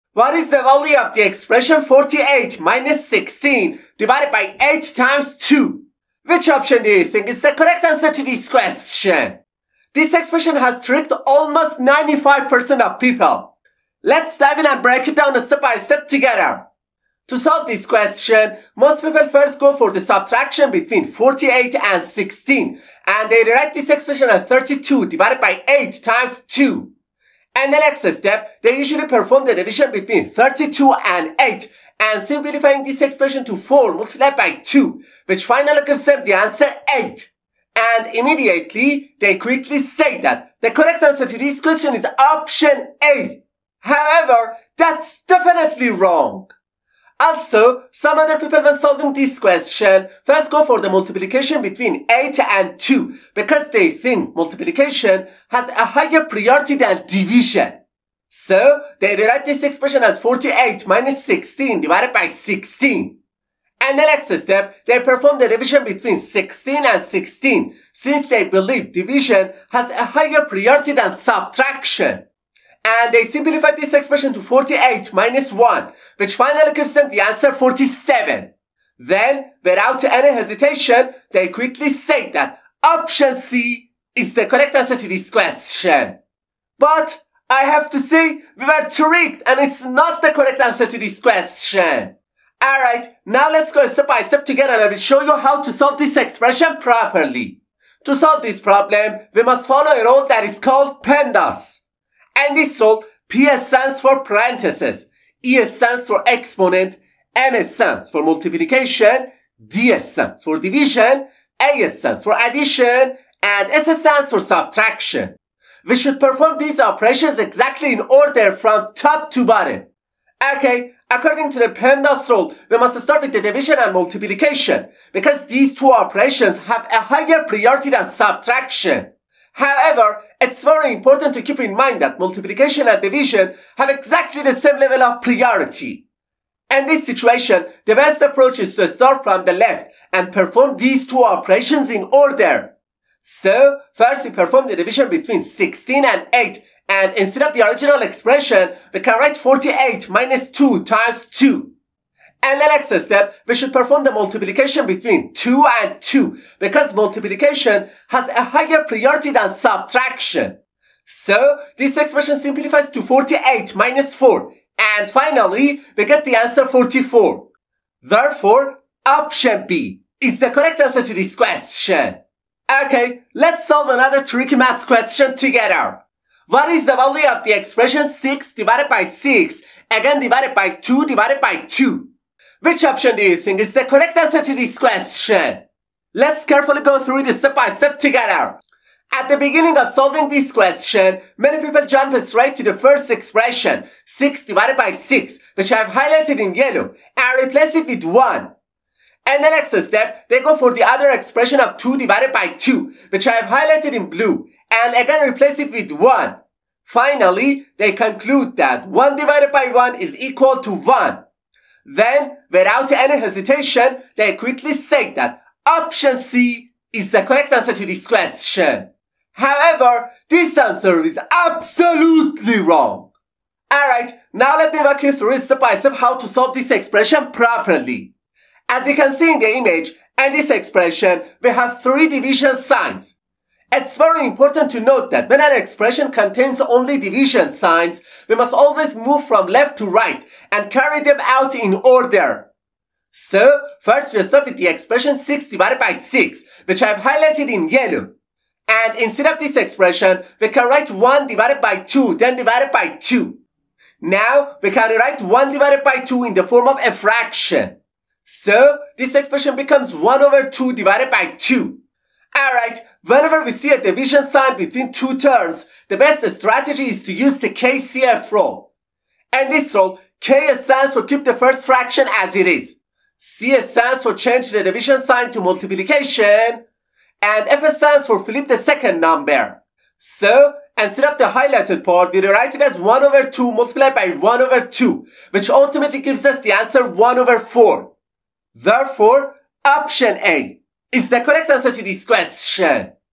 🔥 This Amazing Viral Math Question Is Fooling 95% of People! 🤯
🧠✨ Think you’ve truly mastered PEMDAS?
This seemingly simple math expression is designed to trick your brain and expose the most common mistakes in the order of operations! 😵‍💫➗✖️

If you don’t follow PEMDAS step by step, your final answer could be completely wrong. Parentheses, exponents, multiplication, and division all play a crucial role here — and one wrong move changes everything! ⚠️📊

✅ Apply PEMDAS correctly
✅ See if you’re smarter than most!

Are you ready for the challenge? Let’s solve it together 💪📐🔥